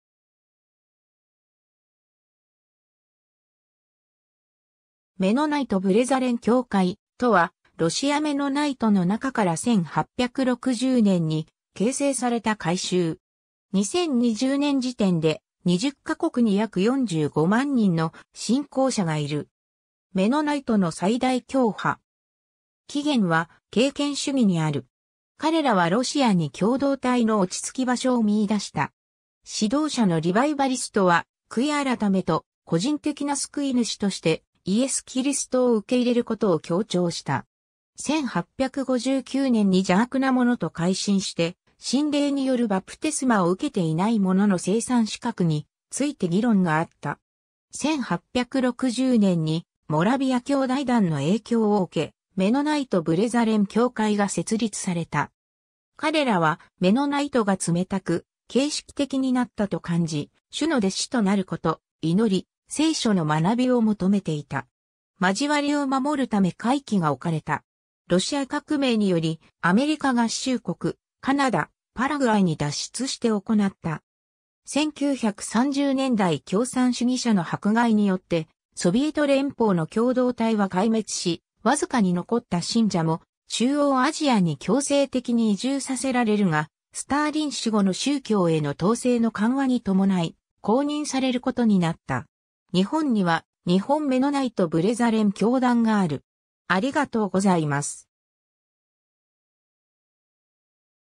メノナイト・ブレザレン教会
メノナイト・ブレザレン教会、（メノナイト・ブレザレンきょうかい、Mennonite Brethren Church）とは、ロシア・メノナイトの中から1860年に形成された会衆。
2020年時点で、20ヶ国に約450,000人の信仰者がいる。
メノナイトの最大教派。
 起源は敬虔主義にある。
彼らはロシアに共同体の落ち着き場所を見い出した。
指導者のリバイバリストは悔い改めと個人的な救い主としてイエス・キリストを受け入れることを強調した。
 1859年に邪悪な者と回心して浸礼によるバプテスマを受けていない者の聖餐資格について議論があった。
1860年にモラヴィア兄弟団の影響を受け、メノナイト・ブレザレン教会が設立された。
彼らはメノナイトが冷たく形式的になったと感じ、主の弟子となること、祈り、聖書の学びを求めていた。
交わりを守るため戒規がおかれた。
 ロシア革命により、アメリカ合衆国、カナダ、パラグアイに脱出して行った。
1930年代共産主義者の迫害によってソビエト連邦の共同体は壊滅し、僅かに残った信者も中央アジアに強制的に移住させられるがスターリン死後の宗教への統制の緩和に伴い公認されることになった。
 日本には日本メノナイト ブレザレン教団がある。